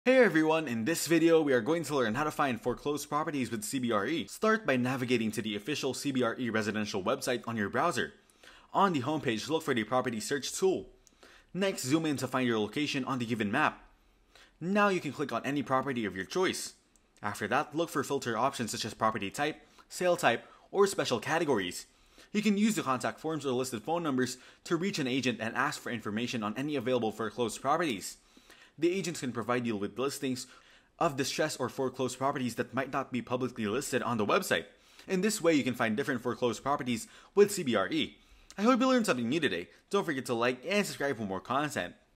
How to FIND Foreclosed PROPERTIES with CBRE | FULL GUIDE 🏢✨
How to Find Foreclosed Properties with CBRE 🏢✨

In this video, we’ll guide you through the process of finding foreclosed properties using CBRE, one of the leading real estate services firms. If you're looking for investment opportunities or affordable housing options, foreclosures can be a great way to enter the real estate market!

🔑 What You’ll Learn:

Understanding Foreclosure Listings: Insights into what to look for in foreclosure listings, including property conditions and potential investment returns.
Tips for Viewing Properties: Advice on scheduling property viewings and what to consider before making an offer.
Financing Foreclosed Properties: Information on financing options and tips for working with lenders when purchasing a foreclosure.

💬 Join the Discussion: Have you purchased a foreclosed property through CBRE? Share your experience or ask questions in the comments below!